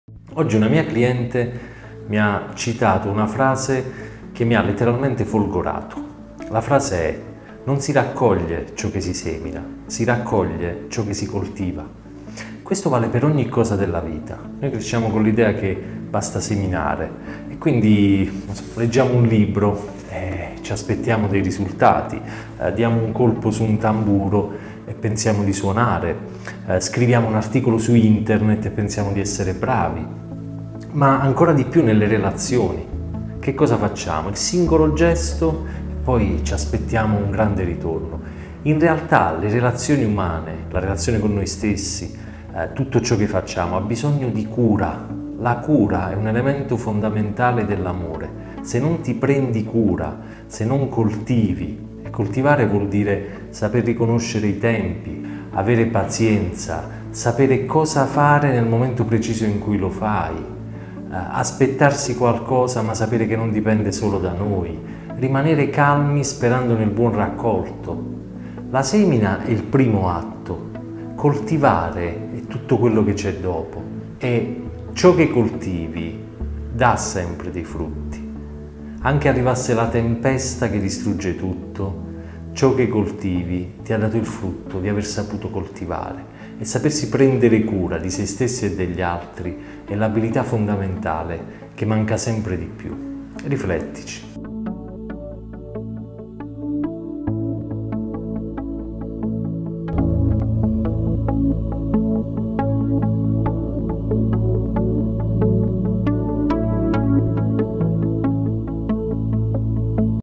I frutti della cura
'Non si raccoglie ciò che si semina, si raccoglie ciò che si coltiva'. Questo vale per ogni cosa della vita. Noi cresciamo con l'idea che basta seminare e quindi leggiamo un libro e ci aspettiamo dei risultati, diamo un colpo su un tamburo e pensiamo di suonare, scriviamo un articolo su internet e pensiamo di essere bravi, ma ancora di più nelle relazioni.

Che cosa facciamo? Il singolo gesto e poi ci aspettiamo un grande ritorno. In realtà le relazioni umane, la relazione con noi stessi, tutto ciò che facciamo ha bisogno di cura! La cura è un elemento fondamentale dell'Amore.

Se non ti prendi cura, se non coltivi - coltivare vuol dire saper riconoscere i tempi, avere pazienza, sapere cosa fare nel momenti preciso in cui lo fai, aspettarsi qualcosa, ma sapere che non dipende solo da noi, rimanere calmi sperando nel buon raccolto.

La semina è il primo atto, coltivare è tutto quello che c'è dopo e ciò che coltivi dà sempre dei frutti, anche arrivasse la tempesta che distrugge tutto, ciò che coltivi ti ha dato il frutto di aver saputo coltivare e sapersi prendere cura di sé stessi e degli altri è l'abilità fondamentale che manca sempre di più.

Riflettici!